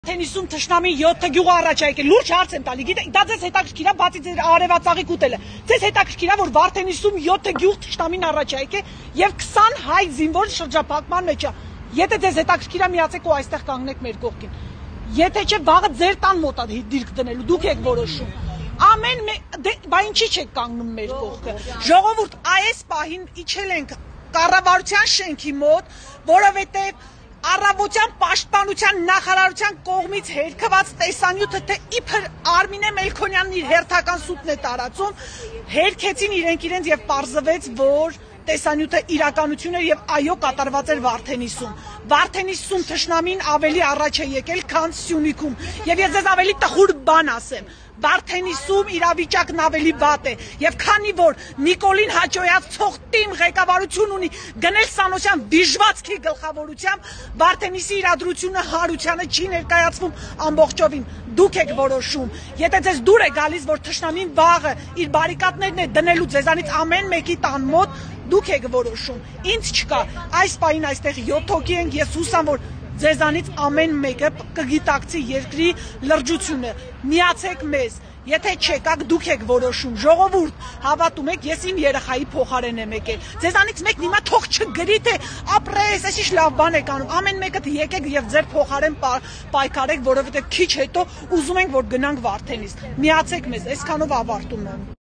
«Թշնամին Գեղարքունիքում 7 գյուղ առաջ է եկել». քաղաքացիները այս պահին կառավարության շենքի դիմաց են
«Թշնամին Վարդենիսի ուղղությամբ 7 գյուղ առաջ է եկել, 20 զինծառայող շրջափակման մեջ է». Գեղարքունիքի մարզի բնակիչ Արմինե Մելքոնյան

Այս պահին քաղաքացիները կառավարության շենքի դիմաց բողոքի ակցիա են իրականացնում՝ պետության սահմաններն անպաշտան թողած Փաշինյանի հրաժարականի պահանջով: